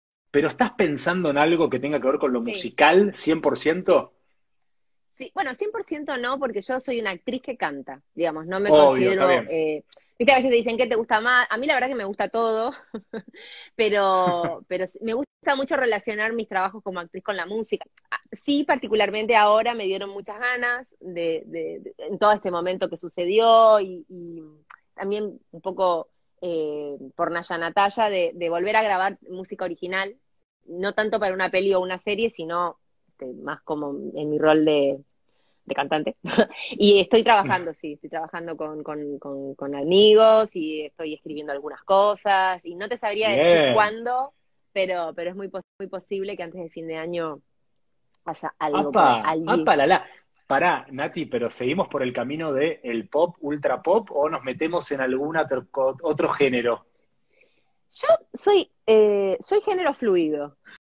Natalia Oreiro - Nueva música en 2020 - Instagram Live con Javi Ponzo para la revista Gente
Natalia Oreiro - Nueva música en 2020 - Instagram Live con Javi Ponzo para la revista Gente (Historias de Instagram de Javi Ponzo, 23/10/2020)